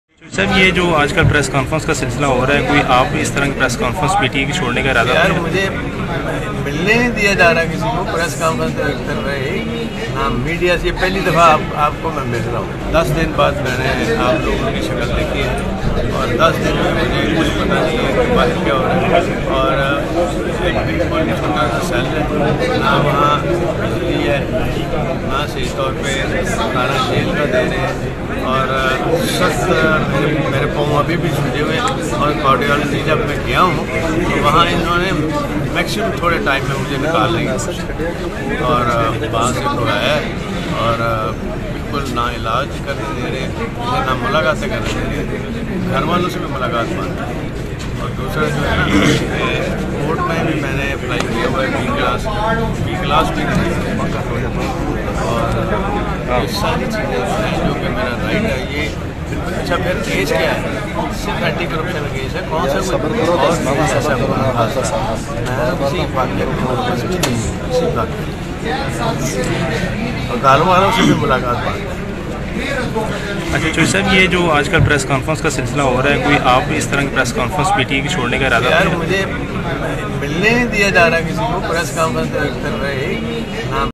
ch pervaiz elahi ki court ma paichi ka doran media talk kia PTI chor rha haia
‏چوہدری پرویز الہی کی کمرہ عدالت سے صحافیوں سے گفتگو

میری طبعیت ٹھیک نہیں ہے،دس روز سے جیل کے چھوٹے سے کمرے میں بند ہوں،جیل کے کمرے میں ہوا کے لیے پنکھا کبھی چلتا ہے کبھی نہیں،گھروں والوں سے ملاقات بھی نہیں کروائی جارہی،پرویز الہی